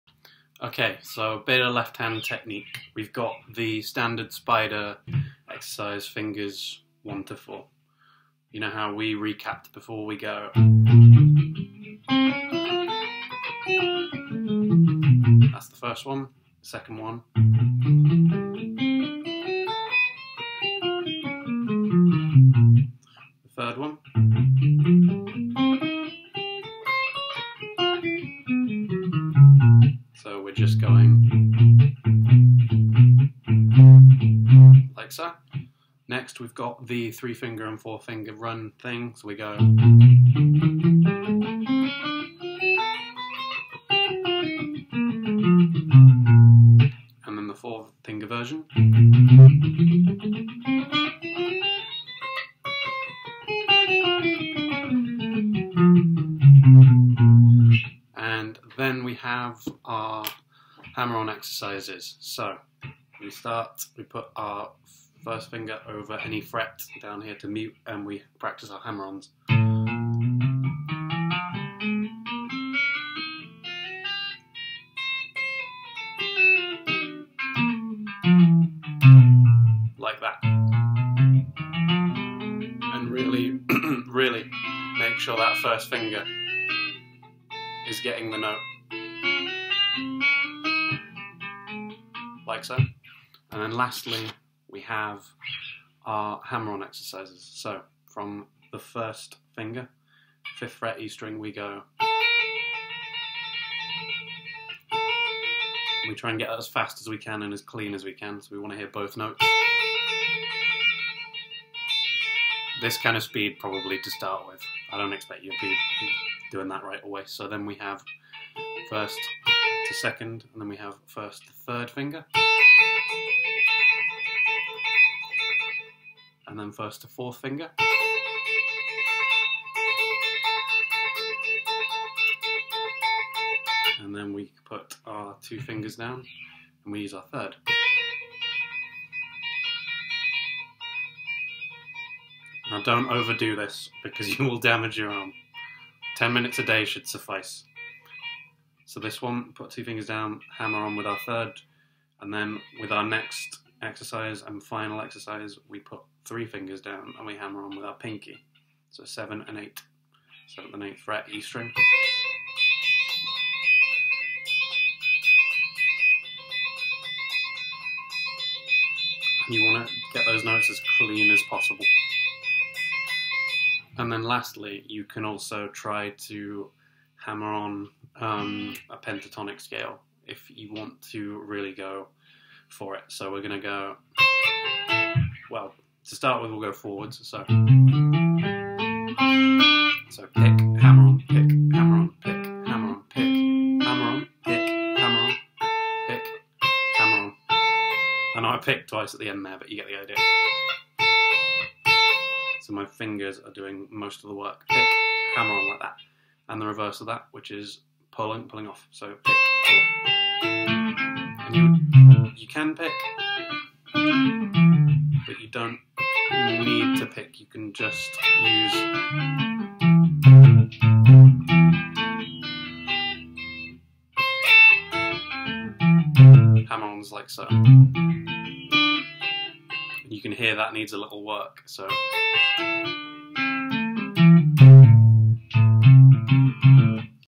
Left Hand Technique- Warm Up Exercises- Spider Exercises, Hammer ons & Pulls offs
Just a quick video on some essential warm up rudiments. These are sure to get your fretting hand into great shape in no time flat!